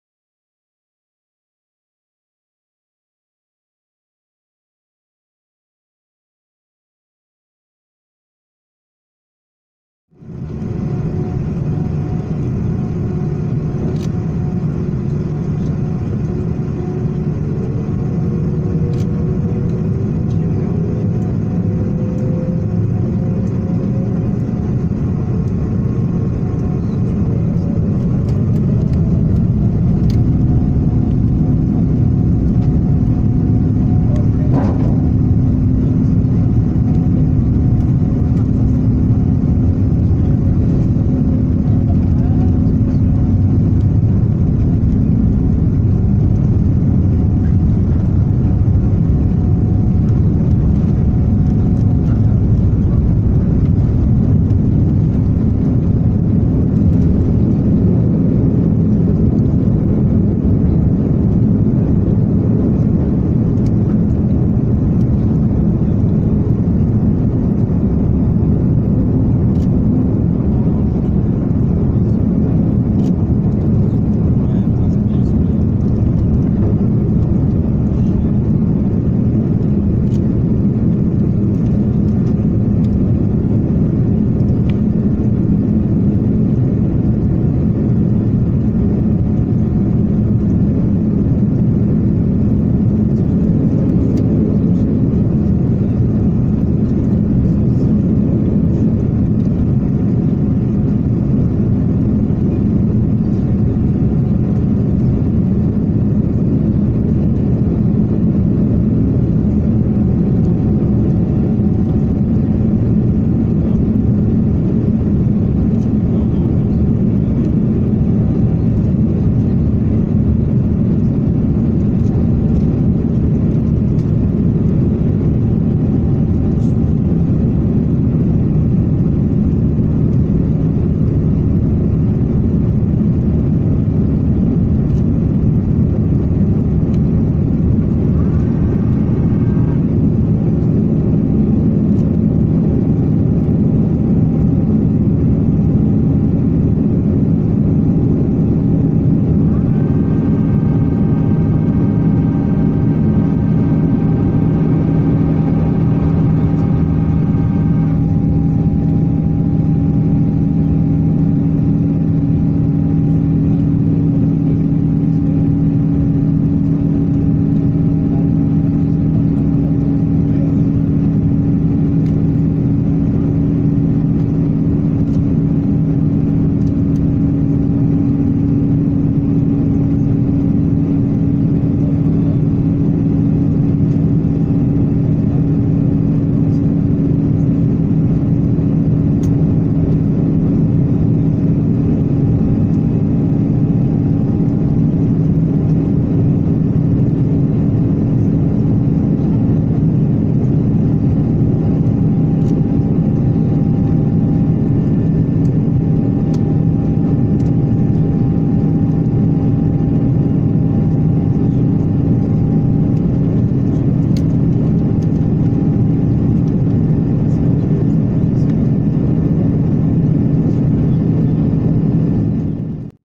Air Mauritius A330-900neo take-off from Reunion Island
Air Mauritius A330-900neo operating flight MK219 from Reunion Island (St. Denis/Roland Garros airport) to Mauritius.

Steep climb out from St. Denis to (briefly!) reach a cruise altitude of 16,000 feet. Airborne for 29 minutes over a distance of 144 miles.